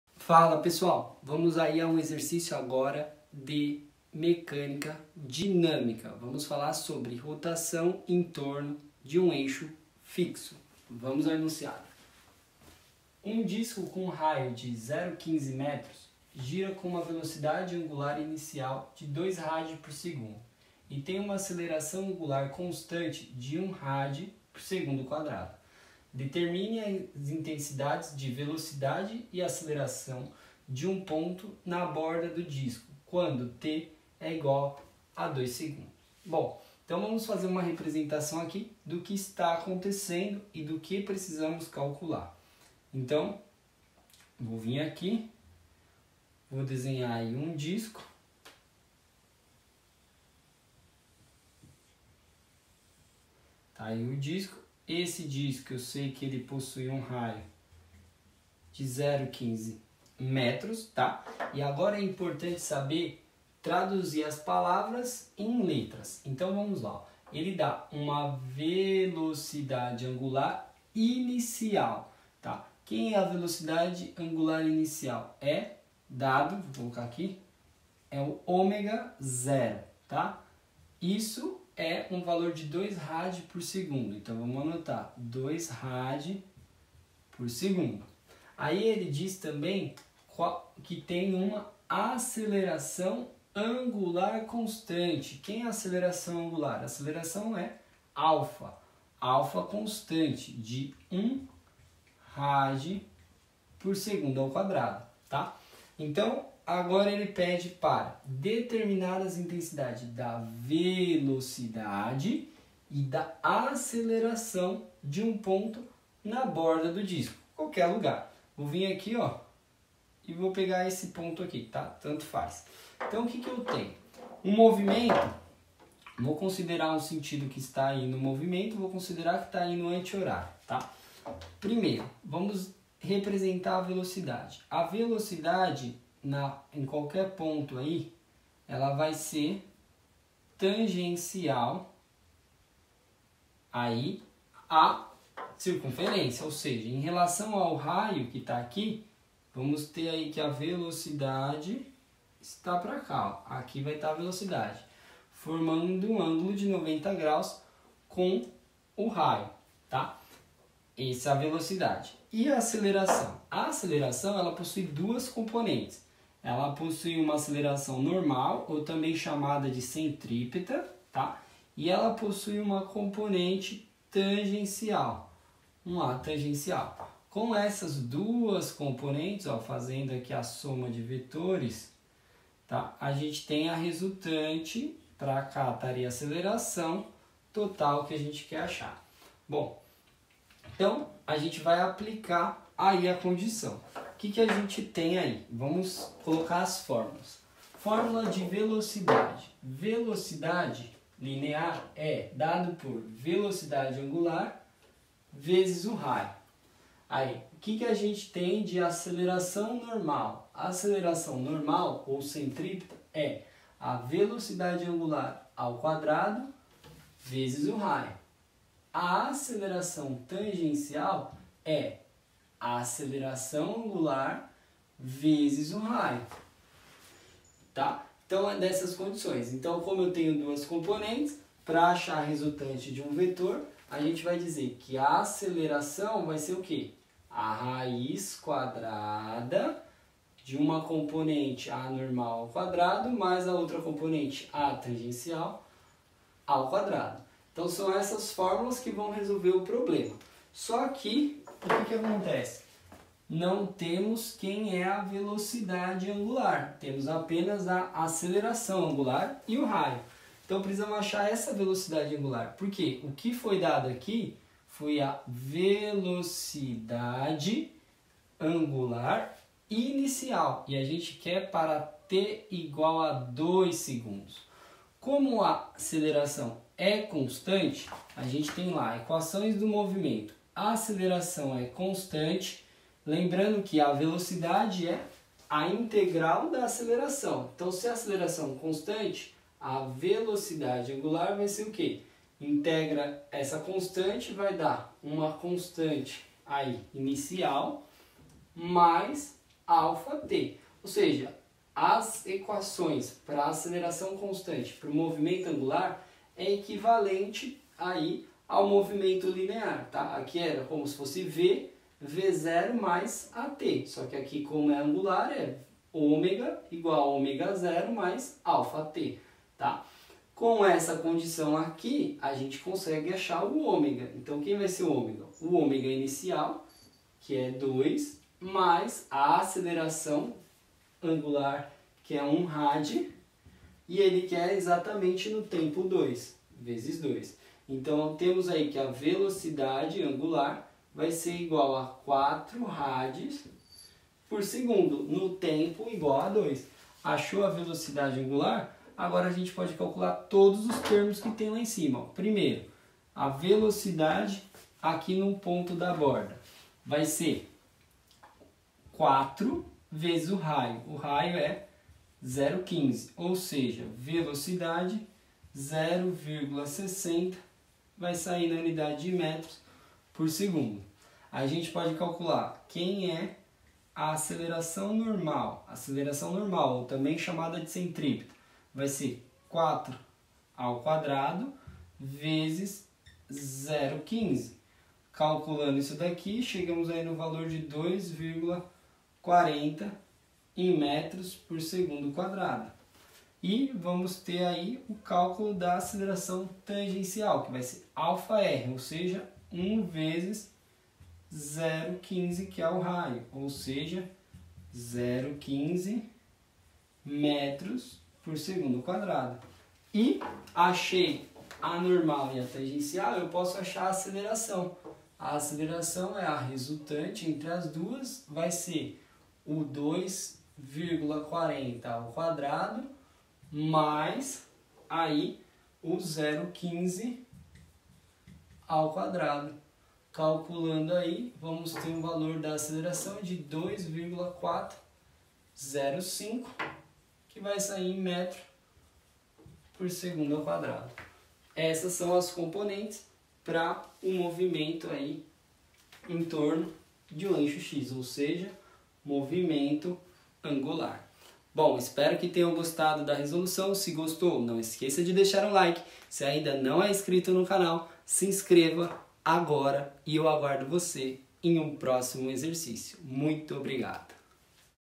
16.1 Cinemática do movimento plano de um corpo rígido
16.1 Um disco com um raio de 0,15 m gira com uma velocidade angular inicial de 2 rad/s e tem uma aceleração angular constante de1 rad/s^2. Determine as intensidades de velocidade e aceleração de um ponto na borda do disco quando t=2s.

Livro: Dinâmica : Mecânica para Engenharia 12ª Edição - Hibbeler